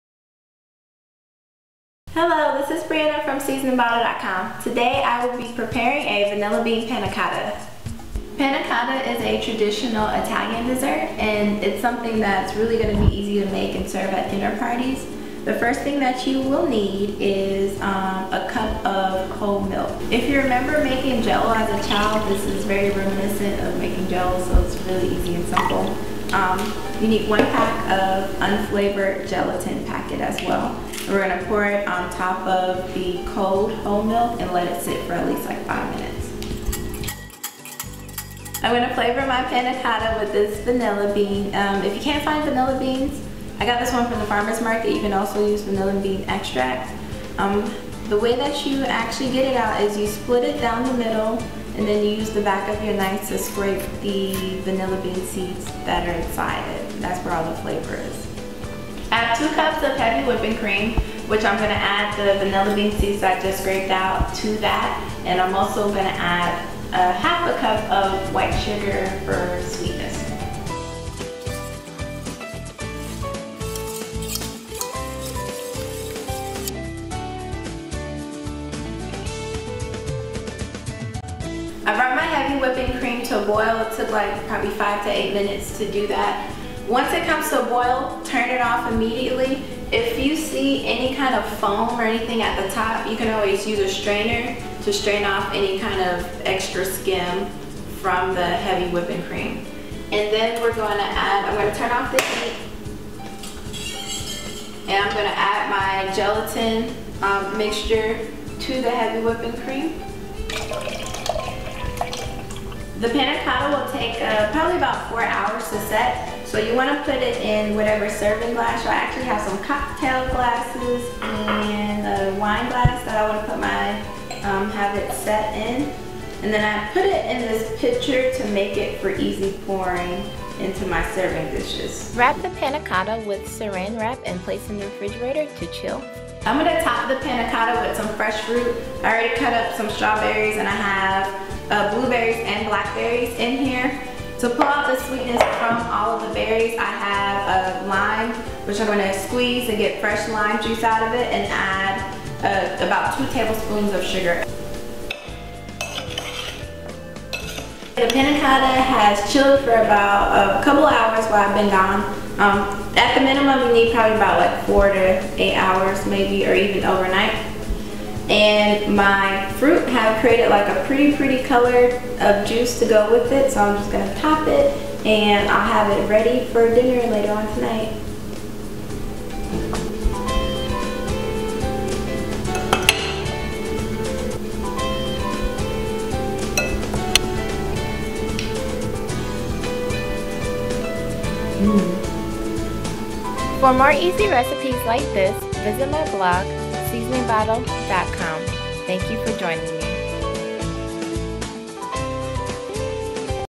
Vanilla Bean Panna Cotta
If you know how to make jello, then you know how to make this Vanilla bean panna cotta. I served this dessert the other day at a small dinner party. It was easy to serve and share. Everyone loved it and was surprised once I revealed how I made it. Try it out! I hope you enjoy this video. I’m getting better at being in front of the camera :)

Ingredients:

1 cup whole milk

2 cups heavy whipping cream

½ cup sugar

1 vanilla bean or 1 teaspoon vanilla extract

1 packet of unflavored gelatin

Directions:

1-In a small mixing bowl combine the whole milk and unflavored gelatin. 2-Combine heavy whipping cream, sugar, and vanilla in a medium saucepan. 3-Bring whipping cream mixture to a boil. 3-Remove from heat once boiled and add milk mixture to the sauce pan. 4-Stir and allow to cool down. 5-Pour panna cotta mixture in various glasses to chill for over 4 hour to over night. 6-Serve with you favorite fruit! Bon Appetite

*Vanilla bean: If you use a vanilla bean, split the bean in half and scrap out seeds from each half. This is where all the vanilla flavor is!